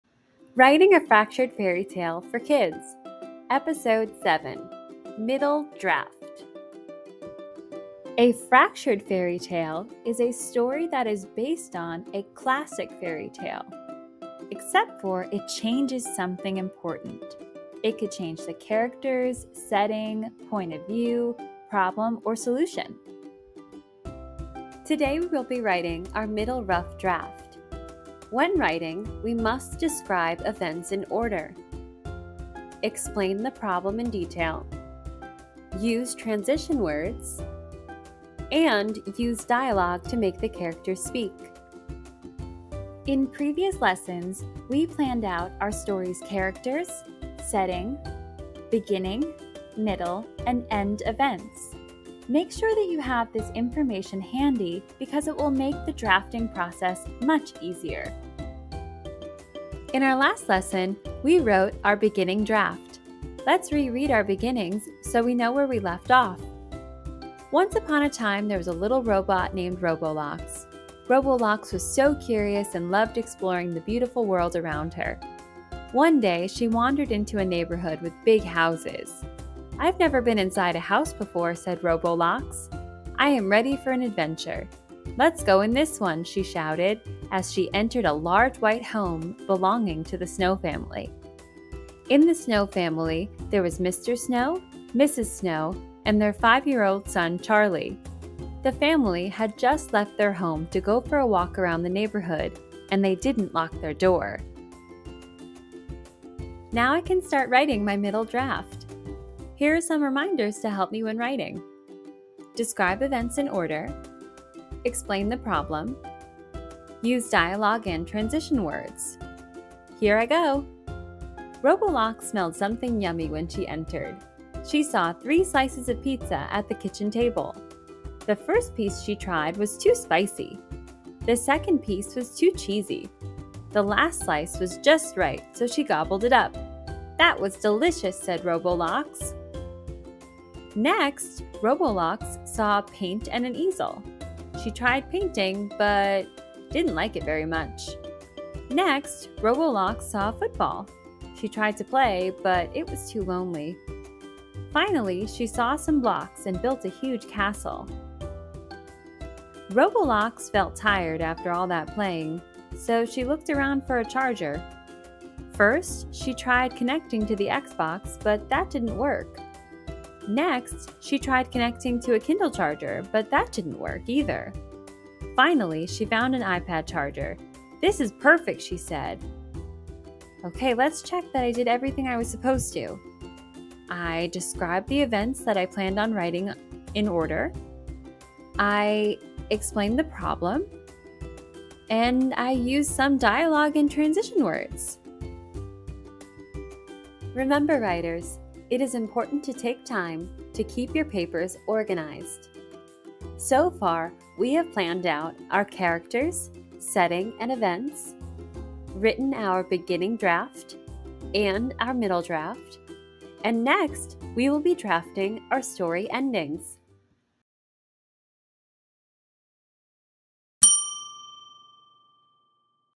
Writing a Fractured Fairy Tale For Kids - Episode 7: Middle Draft
In this episode, children will continue their unit on writing Fractured Fairy Tales by writing a draft for the middle events. This is the seventh video in a 10-part Fractured Fairy Tale writing video series.